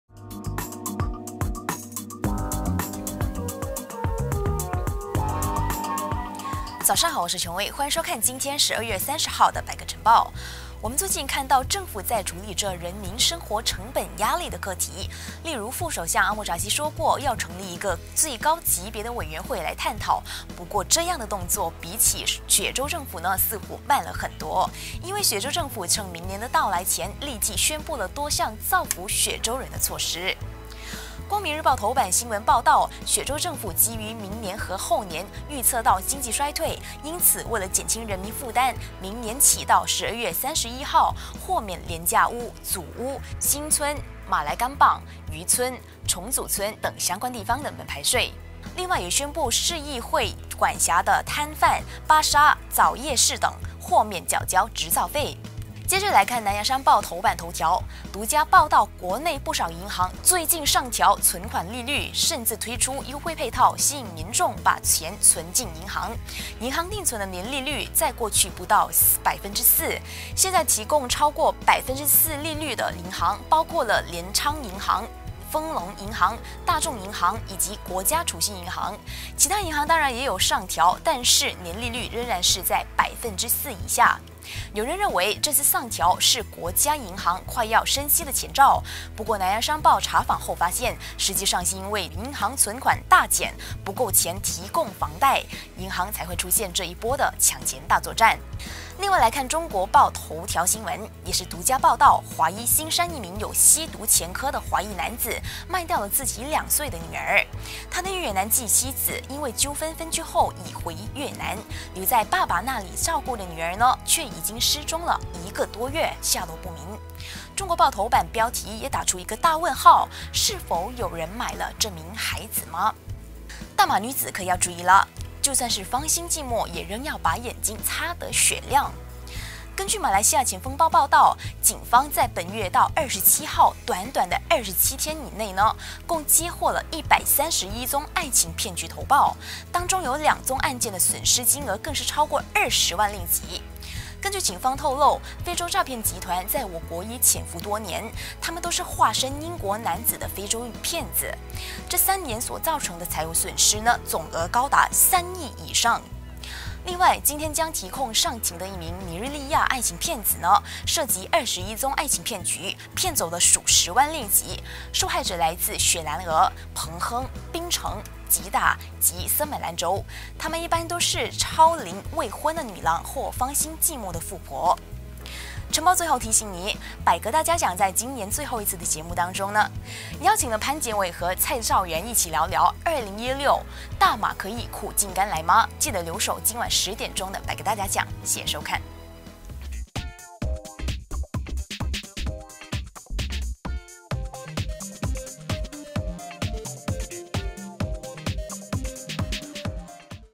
非洲佬斂財不戀人 27天騙131女子
愛情有多盲目？警方透露本月至27日，短短27天內已有131女子陷入愛情騙局，遭斂財金額也自2013年起至今，逐年增加至破億。